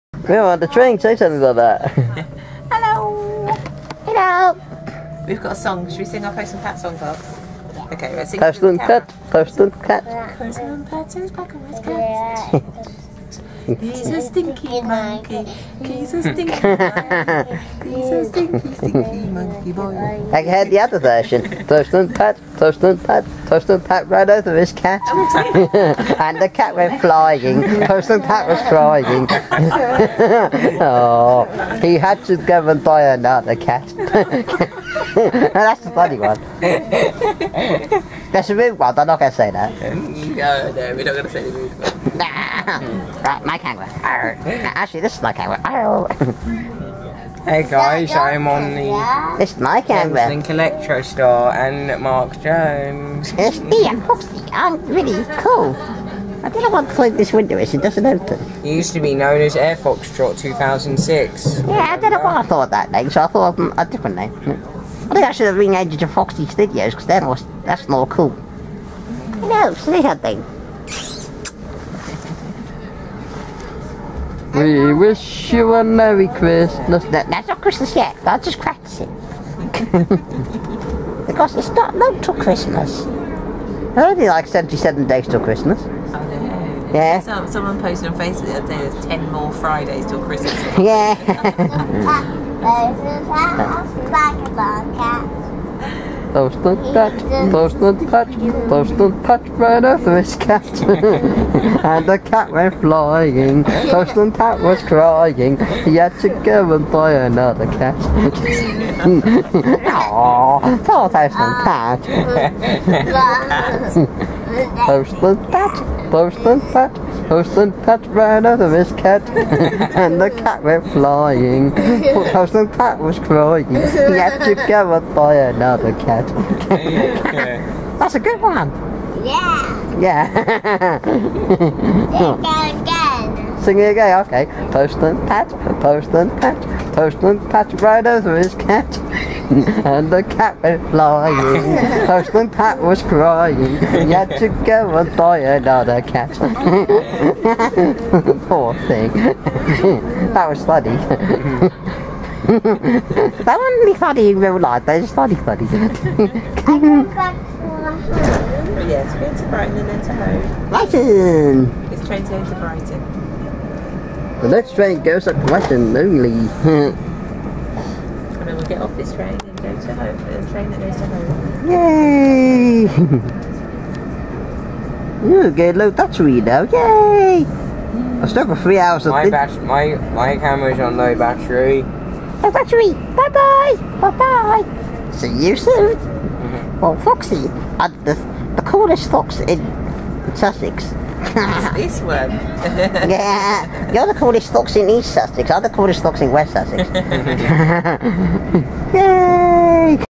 Fun on the Train back to Brighton
Foxy and Foxy313 having fun on the train back to Brighton then Hove and i caught my one home, we had a great first meet up and look forward to meeting again soon.